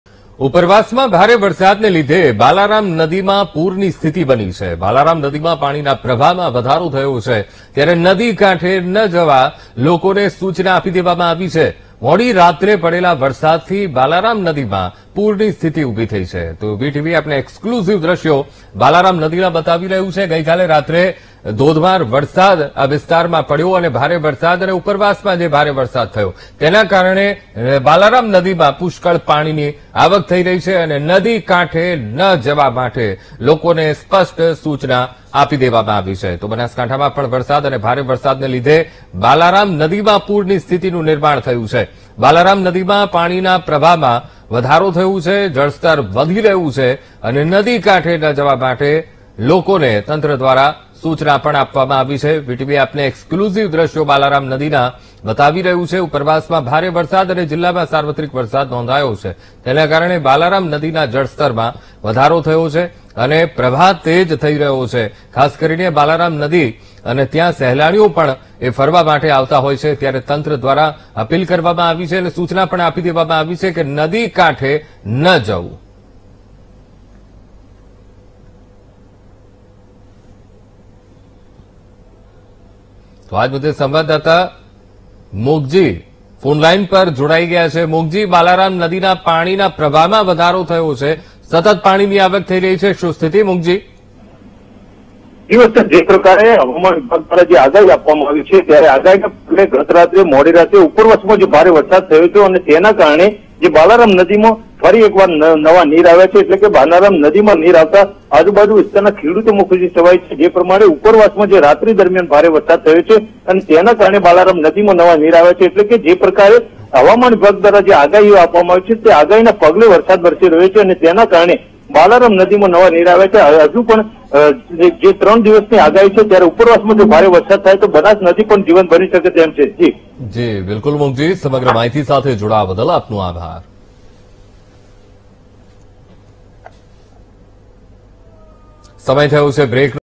Rain in Gujarat: ઉપરવાસમાં ભારે વરસાદના લીધે બાલારામ નદીમાં પૂર | VTV GUJARATI
Rain in Gujarat: બાલારામ નદીમાં પાણીના પ્રવાહમાં વધારો, નદી કાંઠે ન જવા લોકોને સૂચના   | VTV GUJARATI